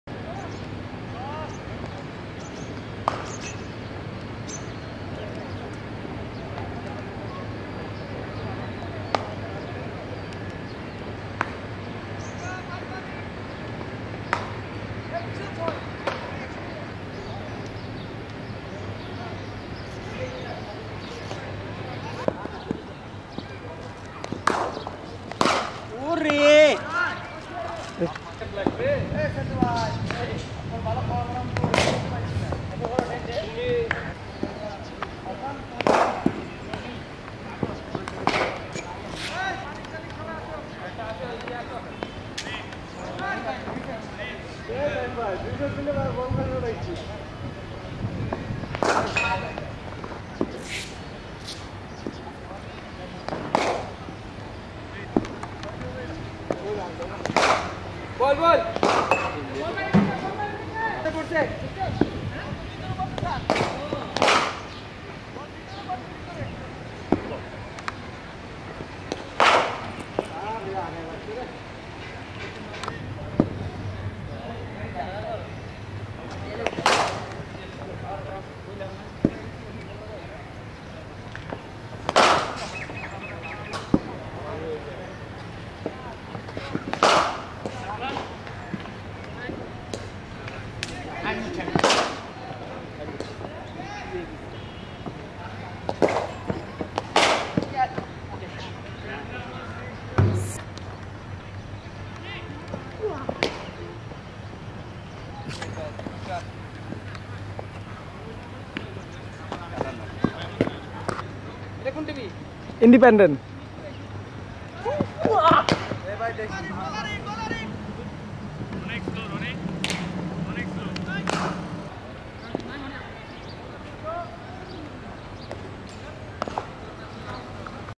Fatullah Khan Shaheb Osman Ali Stadium (ফতুল্লা খান সাহেব ওসমান আলী স্টেডিয়ামে ক্রিকেটারদের অনুশীলন)
ফতুল্লা খান সাহেব ওসমান আলী স্টেডিয়ামে বাংলাদেশ জাতীয় দলের ক্রিকেটার সোহাগ গাজী ও অন্যান্য ক্রিকেটারদের অনুশীলন।